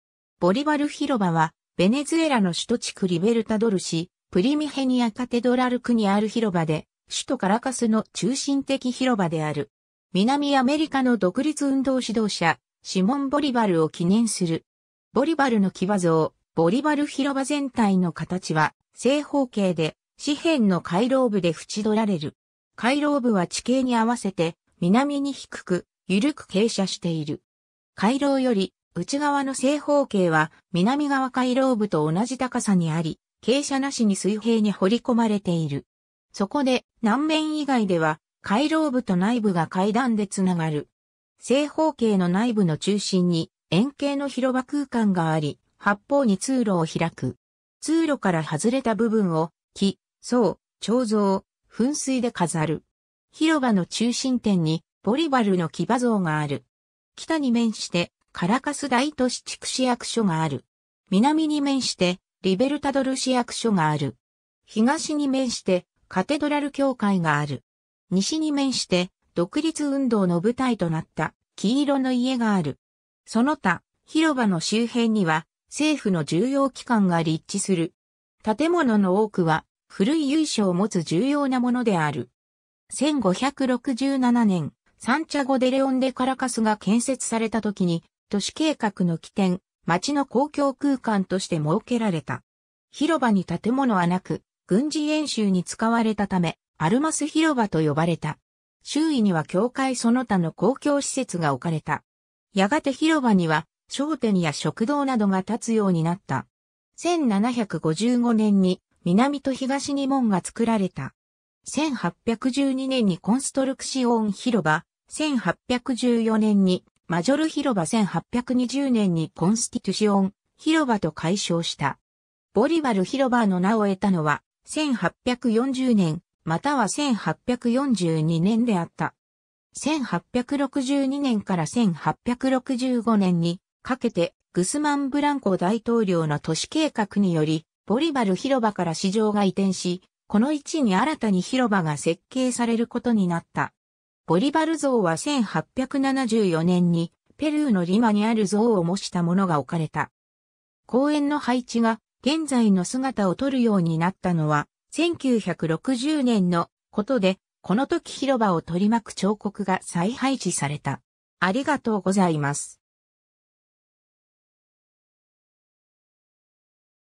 ボリバル広場 (カラカス)
シモン・ボリバル
ボリバル広場（スペイン語: Plaza Bolívar）は、ベネズエラの首都地区リベルタドル市プリミヘニアカテドラル区にある広場で、首都カラカスの中心的広場である。
南アメリカの独立運動指導者シモン・ボリバルを記念する。
 ボリバルの騎馬像（2004年6月） ボリバル広場全体の形は正方形で、四辺の回廊部で縁取られる。
回廊部は地形にあわせて南に低く、緩く傾斜している。
回廊より内側の正方形は南側回廊部と同じ高さにあり、傾斜なしに水平に掘り込まれている。
そこで、南面以外では回廊部と内部が階段でつながる。
正方形の内部の中心に円形の広場空間があり、八方に通路を開く。
通路から外れた部分を木、草、彫像、噴水で飾る。
広場の中心点にボリバルの騎馬像がある。
 北に面してカラカス大都市地区市役所がある。
南に面してリベルタドル市役所がある。
東に面してカテドラル教会がある。
西に面して独立運動の舞台となった「黄色の家」がある。
その他、広場の周辺には、政府の重要機関が立地する。
建物の多くは古い由緒を持つ重要なものである。
 1567年、サンチャゴデレオンデカラカス（後のカラカス）が建設されたときに、都市計画の起点、町の公共空間として設けられた。
広場に建物はなく、軍事演習に使われたため、アルマス広場 (Plaza de Armas、武器広場) と呼ばれた。
周囲には教会その他の公共施設がおかれた。
 やがて広場には商店や食堂などが立つようになった。
1755年に、南と東に門が作られた。
1812年にコンストルクシオン広場 (Plaza de Construcción、建設広場)、1814年にマジョル広場 (Plaza Mayor、大広場) 、1820年にコンスティトゥシオン広場 (Plaza de Constitución、憲法広場) と改称した。
ボリバル広場の名を得たのは、1840年（広場の解説板による）、または1842年 (Elschnig 2003、スペイン語版ウィキペディアによる) であった。
 1862年から1865年にかけて、グスマン・ブランコ大統領の都市計画により、ボリバル広場から市場が移転し、この位置に新たに広場が設計されることになった。
ボリバル像は1874年に、ペルーのリマにある像を模したものが置かれた。
 公園の配置が現在の姿をとるようになったのは、1960年のことで、このとき広場をとりまく彫刻が再配置された。